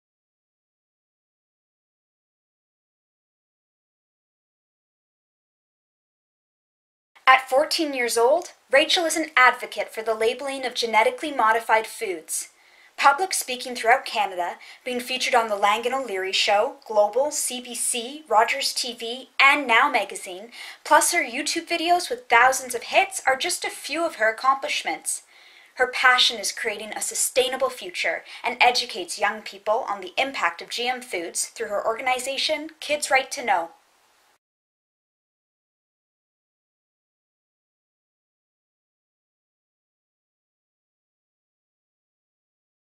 Rachel Parent - GMO Activist
We're profiling Rachel Parent on our Road to Possible series!